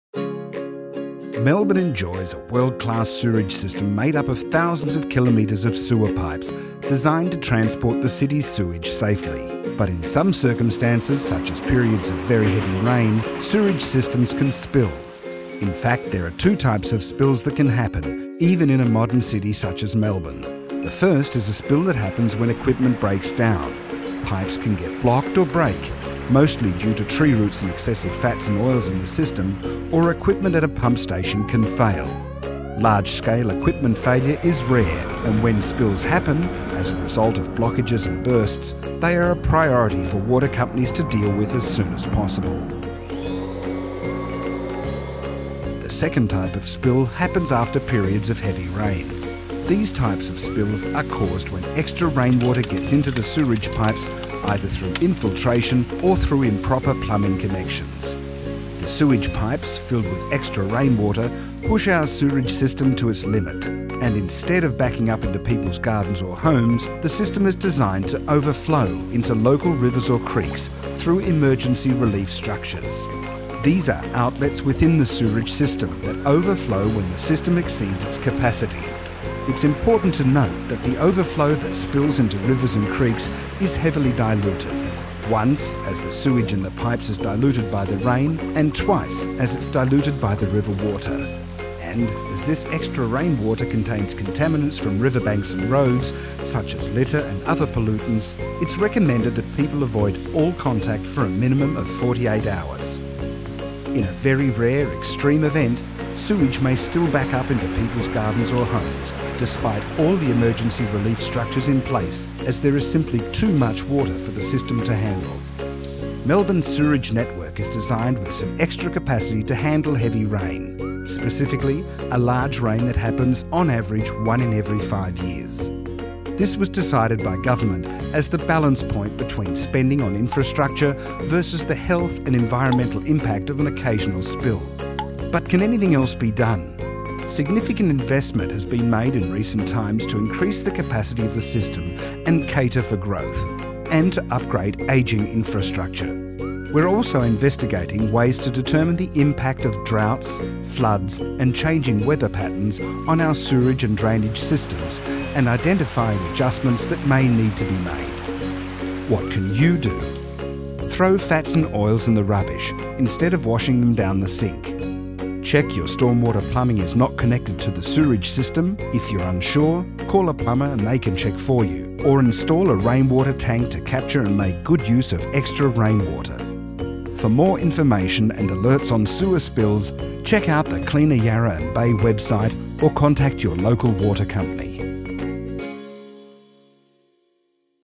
Sewer Spills
Melbourne has a world class sewage system, but sewer spills can still occur.  We are working on ways to further prevent spills by increasing the capacity of the system to cater for growth and upgrading aging infrastructure.  We are also investigating how changing weather patterns can impact the system and identify adjustments that may need to be made.

There are also things you can do to help keep our pipes and sewers in working condition.